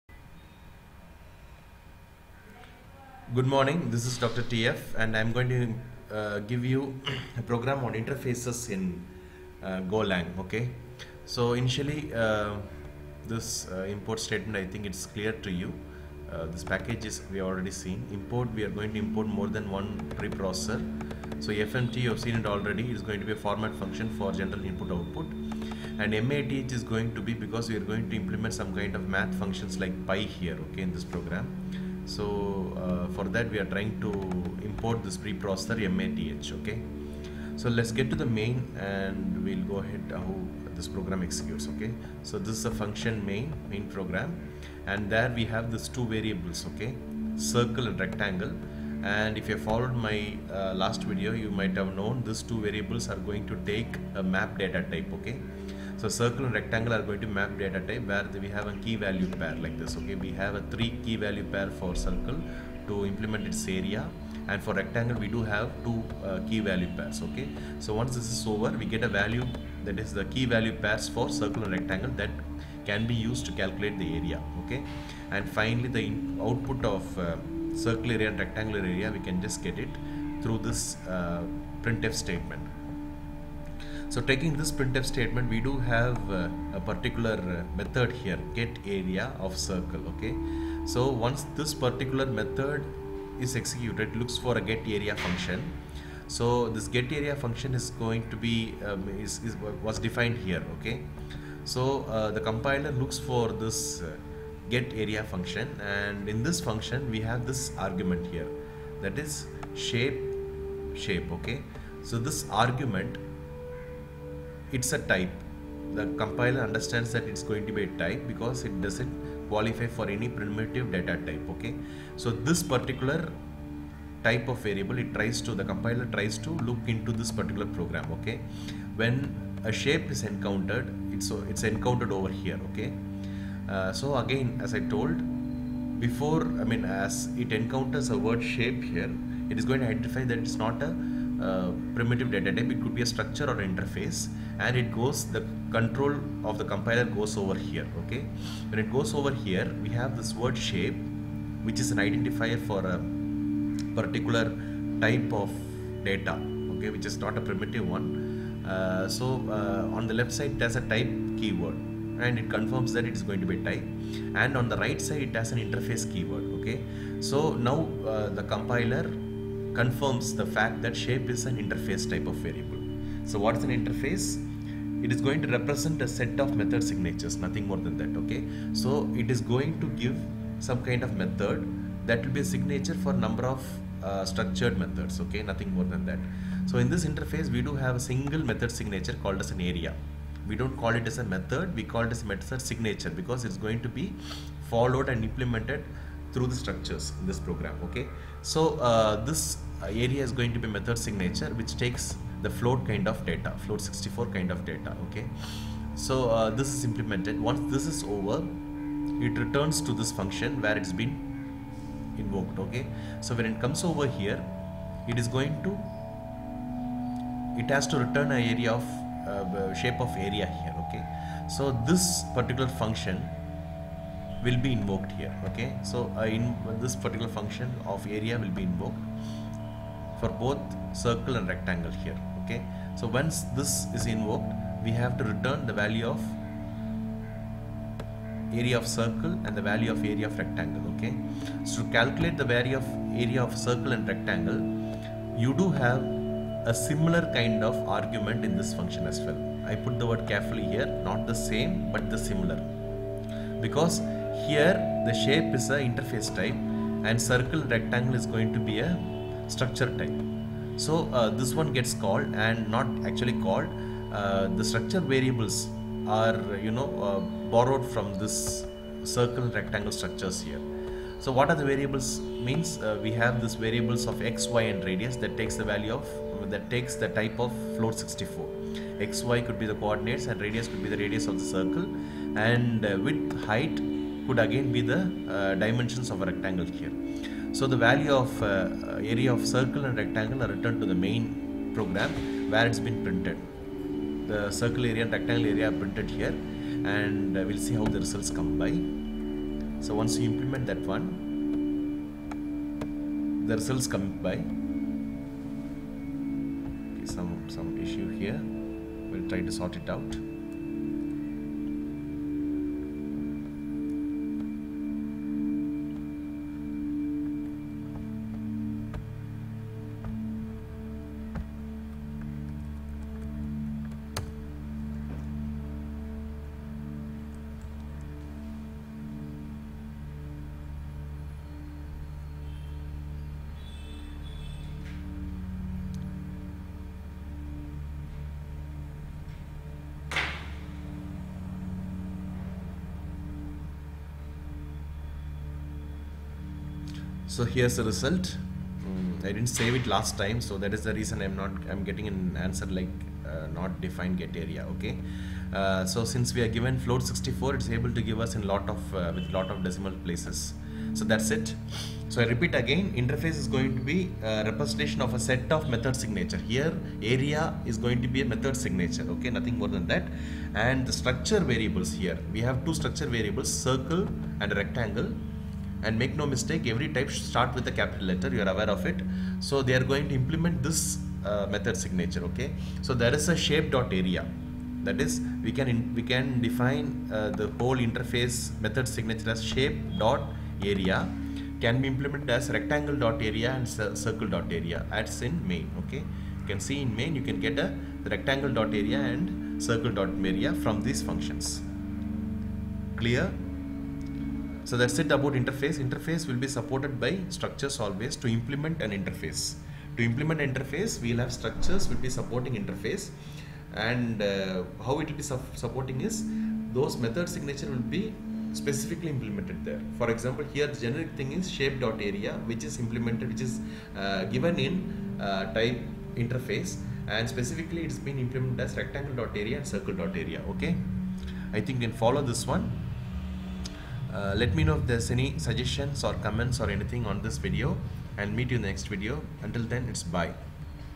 Video 11: Google's GoLang - Interfaces & method signatures
Go is an open source programming language that makes it easy to build simple, reliable, and efficient software.The Go programming language is an open source project to make programmers more productive.It's a fast, statically typed, compiled language that feels like a dynamically typed, interpreted language.

In this basic 60 minute course, you will get acquainted with the Google's GoLang.
Download the software :
(For windows)